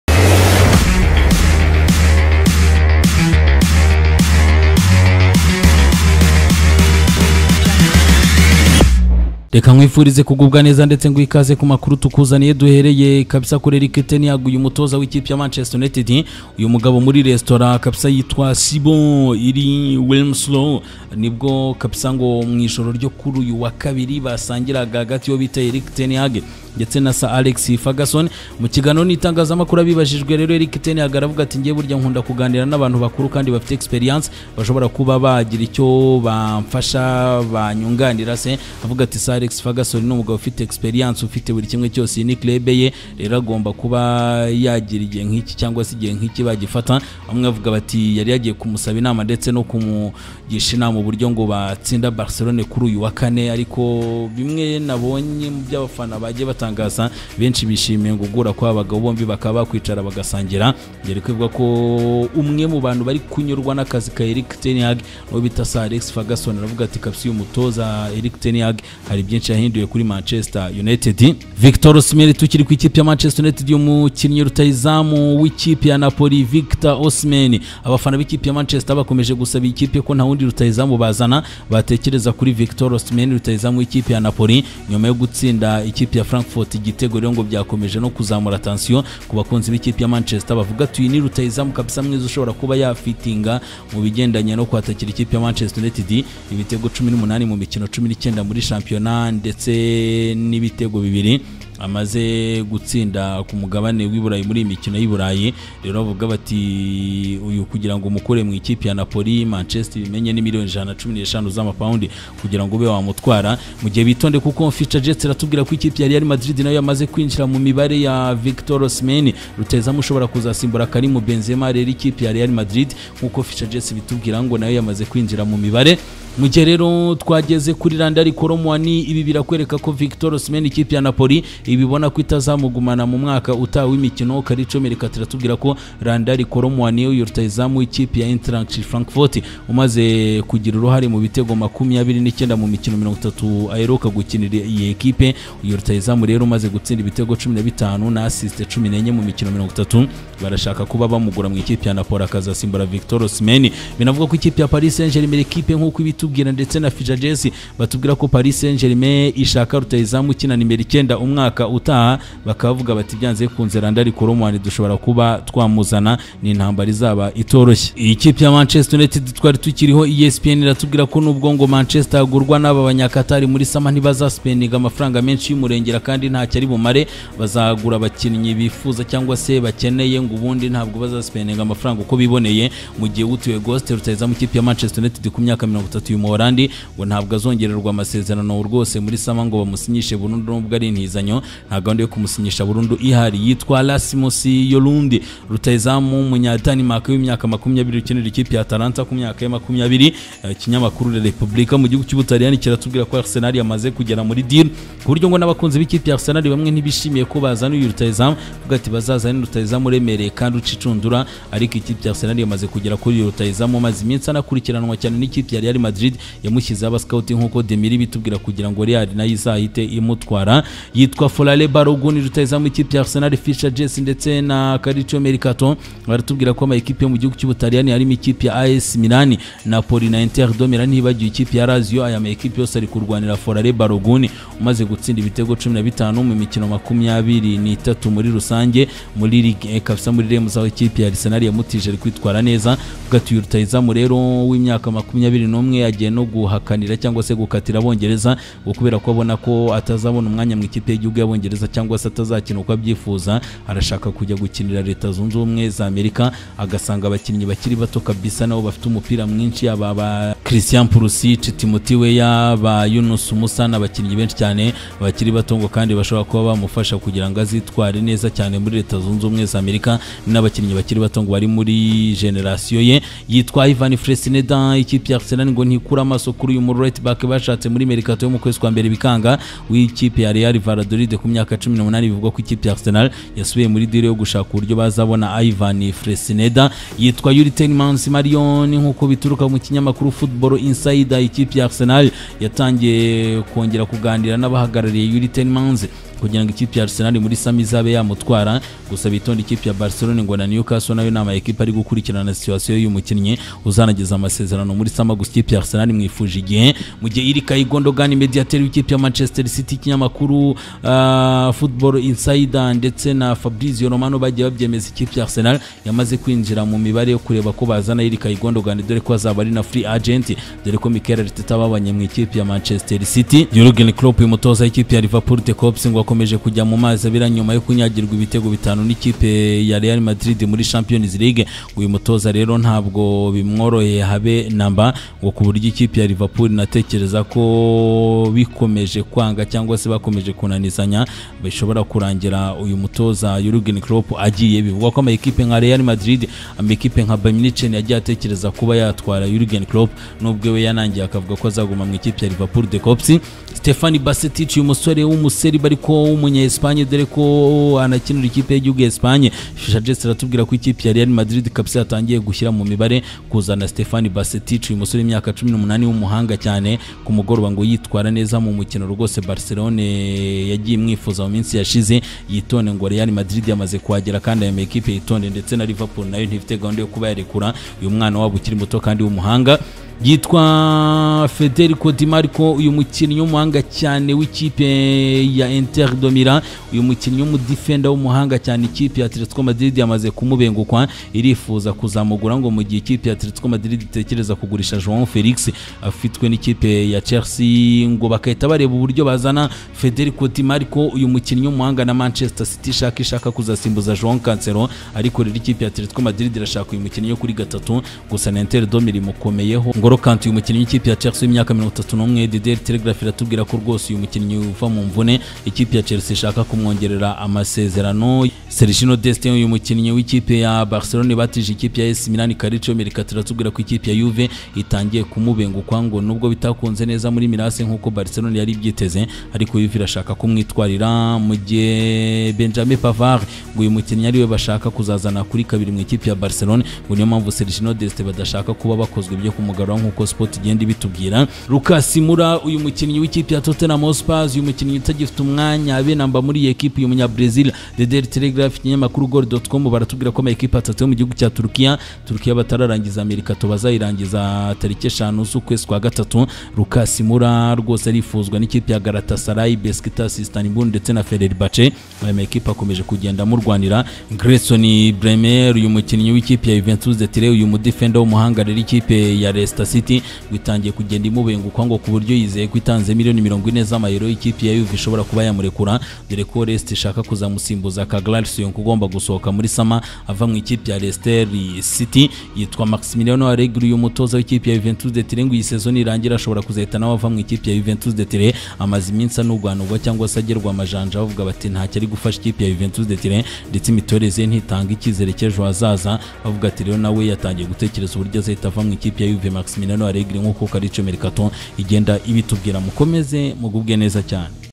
Arsenal irakurikirana Gundogan na Højlund //Osihmen na Man Utd byashyushye // Klopp ntago yorohewe
Ntucikwe namakuru agezweho I Burayi nahandi hose kwisi